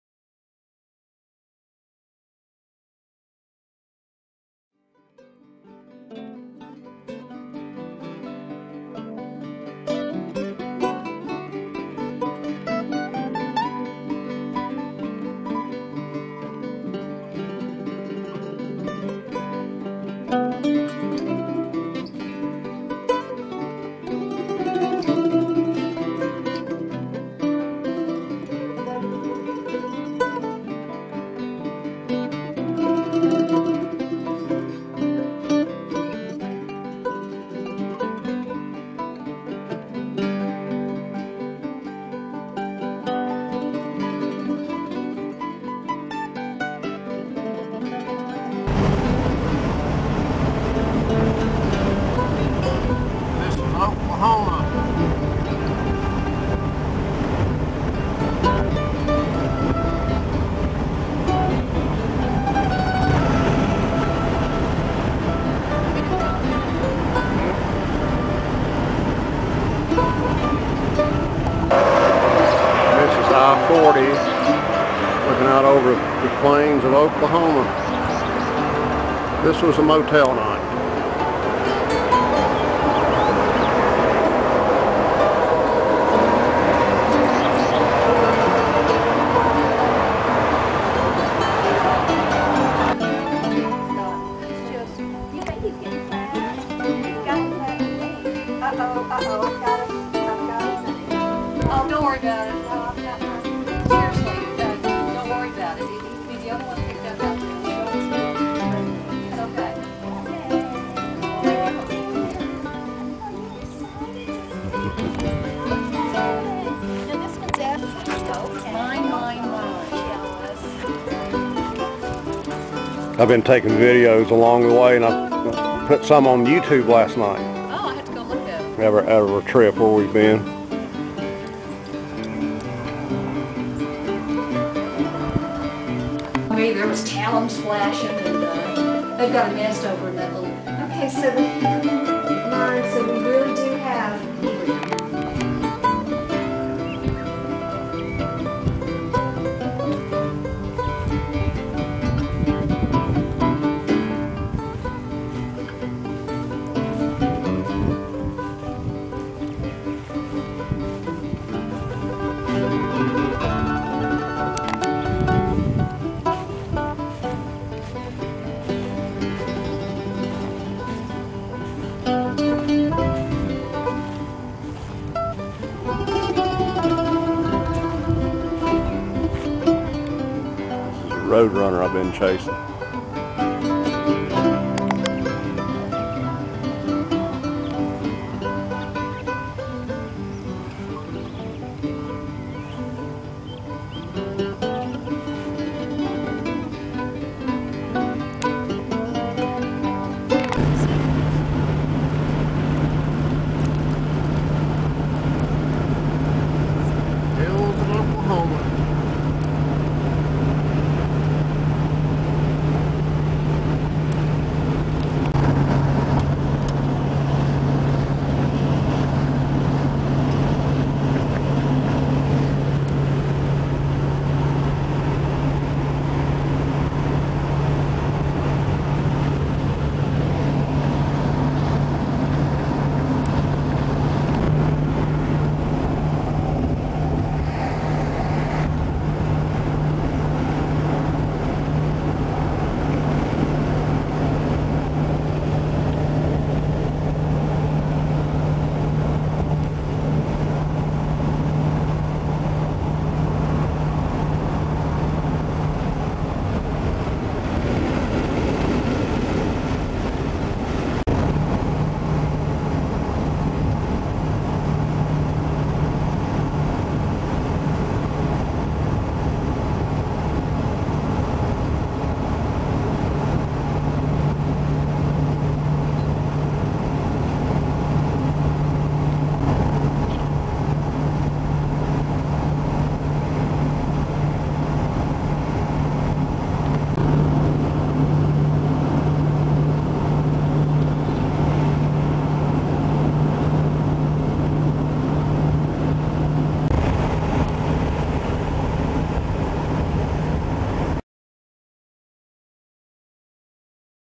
West Road Trip, Days 3-4
Days 3 and 4 of a trip out West, mostly in Oklahoma.  Stopped to see some friends.   Day 5 is Palo Duro Canyon State Park in Texas.   Check it out, more to come